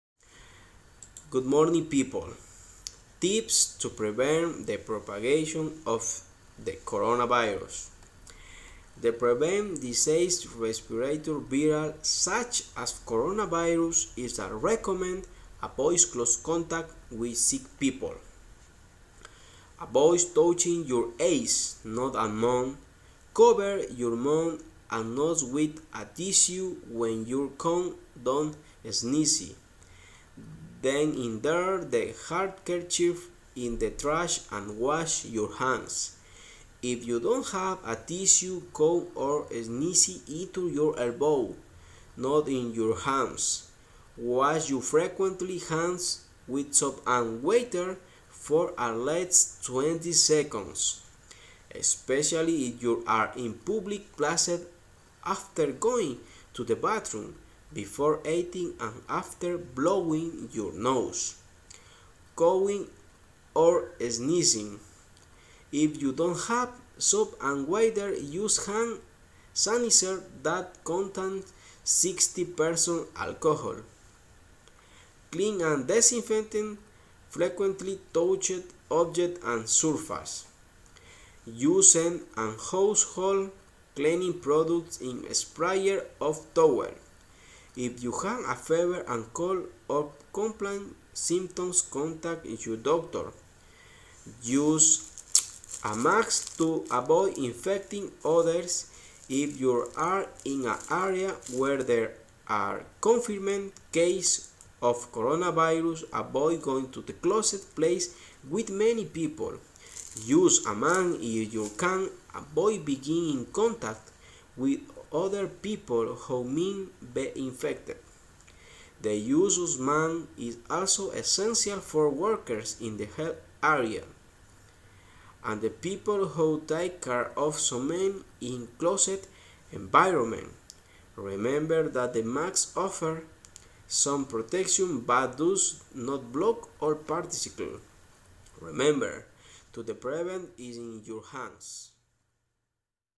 Coronavirus prevention measures
Semi presencial students telling us about measures we should take to help prevent the Coronavirus.